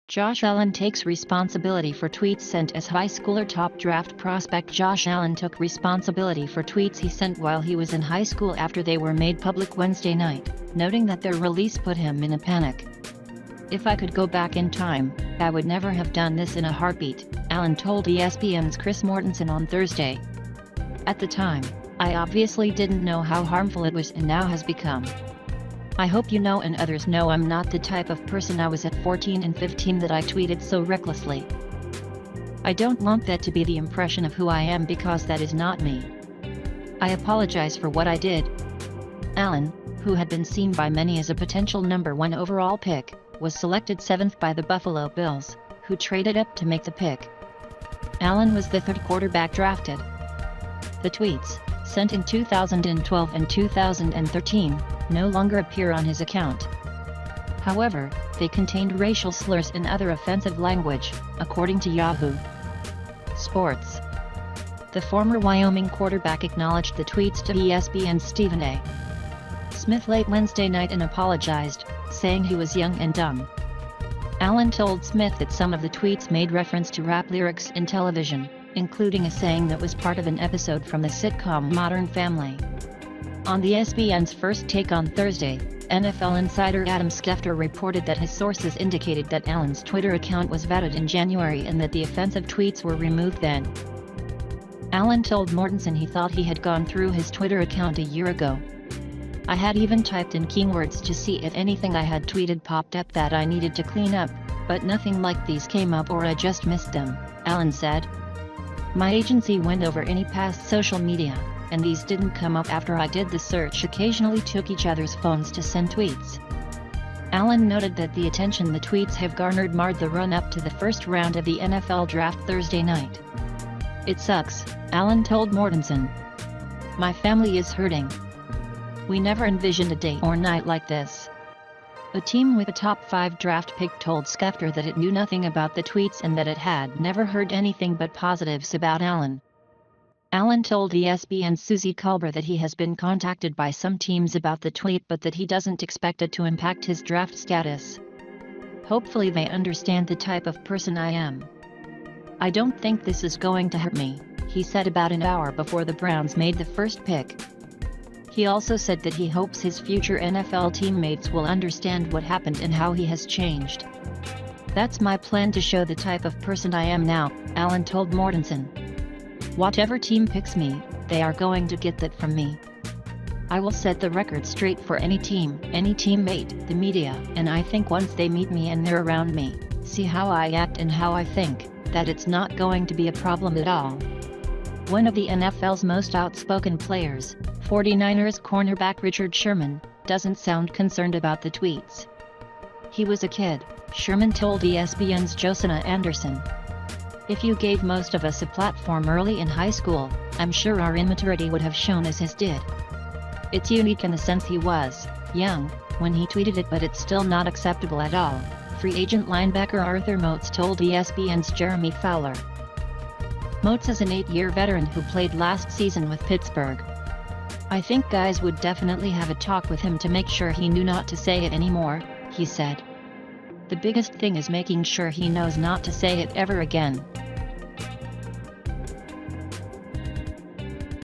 Josh Allen takes responsibility for tweets sent as high schooler
Top draft prospect Josh Allen took responsibility for tweets he sent while he was in high school after they were made public Wednesday night, noting that their release put him "in a panic."
"If I could go back in time, I would never have done this in a heartbeat," Allen told ESPN's Chris Mortensen on Thursday. "At the time, I obviously didn't know how harmful it was and now has become.
"I hope you know and others know I'm not the type of person I was at 14 and 15 that I tweeted so recklessly. ... I don't want that to be the impression of who I am because that is not me. I apologize for what I did."
Allen, who had been seen by many as a potential No. 1 overall pick, was selected seventh by the Buffalo Bills, who traded up to make the pick. Allen was the third quarterback drafted.
The tweets, sent in 2012 and 2013, no longer appear on his account. However, they contained racial slurs and other offensive language, according to Yahoo! Sports.
The former Wyoming quarterback acknowledged the tweets to ESPN's Stephen A. Smith late Wednesday night and apologized, saying he was young and dumb.
Allen told Smith that some of the tweets made reference to rap lyrics and television, including a saying that was part of an episode from the sitcom "Modern Family."
On ESPN's First Take on Thursday, NFL Insider Adam Schefter reported that his sources indicated that Allen's Twitter account was vetted in January and that the offensive tweets were removed then